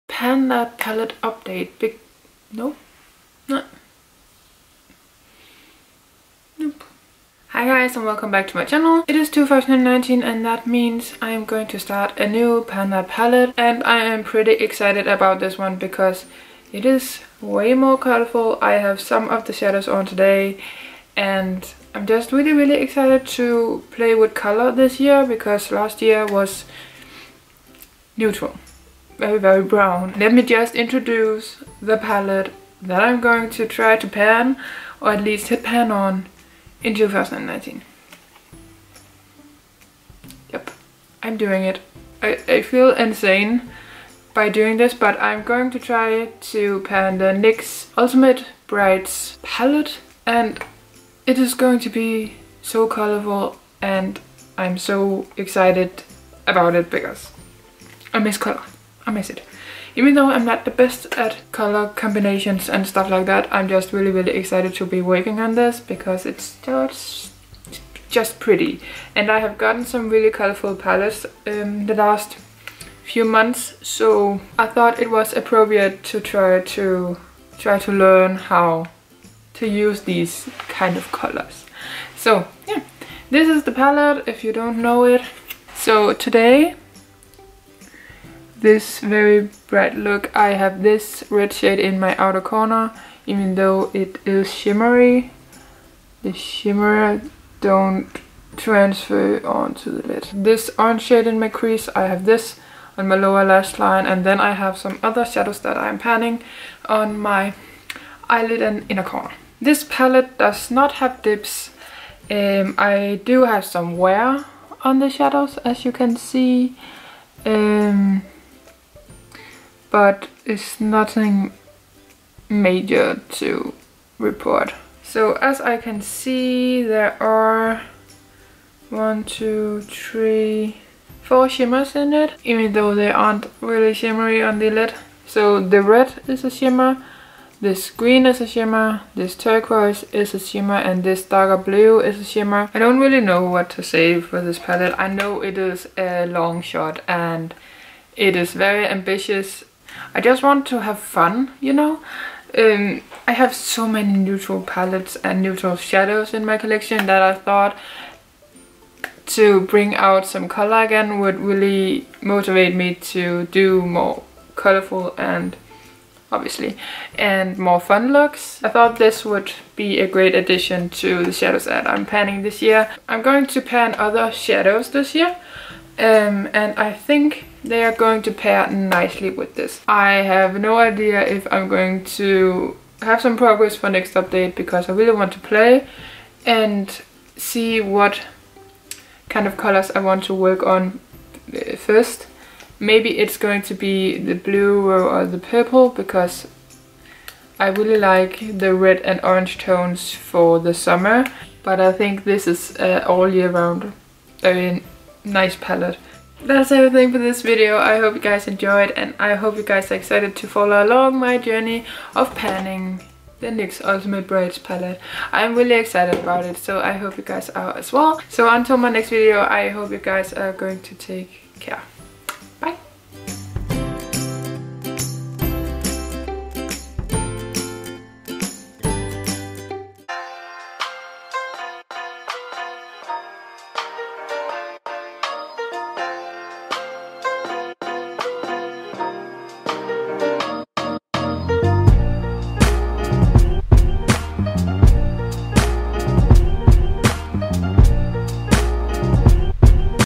PAN THAT PALETTE 2019 // INTRO
For 2019 i am going to be panning the NYX Ultimate Brights palette! Insane - i know. But i have been craving color and this is the perfect way to get color into my looks.A huge goal is to be GOOD at combining colors and HAVE FUN. So Hitting pan in all shades would be amazing. Panning the hole thing would be MINDBLOWING. But i am going to do my best!
Do you know anyone who have panned this palette? Let me know if your answer is yes!

C A M E R A: Sony Alpha A5100 systemkamera + 16-50 mm objektiv
M U S I C:
 - YOUTUBE AUDIOLIBRARY